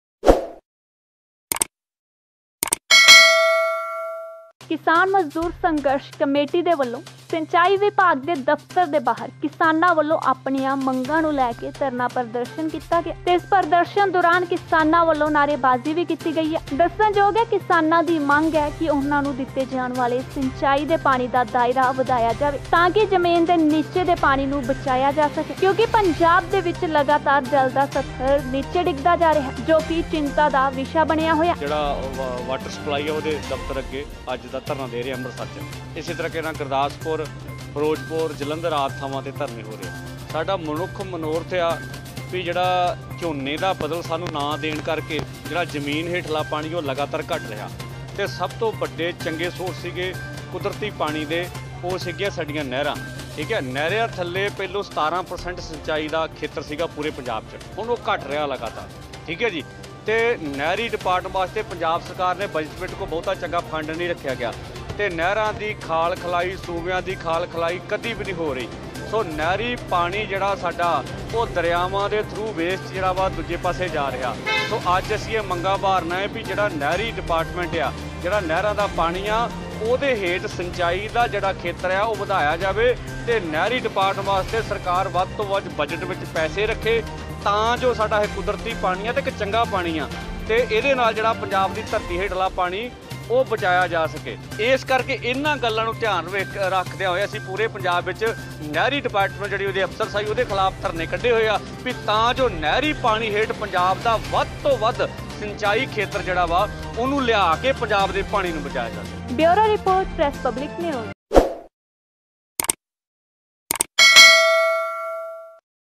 ਸਿੰਚਾਈ ਵਿਭਾਗ ਦਫ਼ਤਰ ਦੇ ਬਾਹਰ ਕਿਸਾਨਾਂ ਨੇ ਕੀਤਾ ਧਰਨਾ ਪ੍ਰਦਰਸ਼ਨ || Press Public
ਸਿੰਚਾਈ ਵਿਭਾਗ ਦਫ਼ਤਰ ਦੇ ਬਾਹਰ ਕਿਸਾਨਾਂ ਨੇ ਕੀਤਾ ਧਰਨਾ ਪ੍ਰਦਰਸ਼ਨ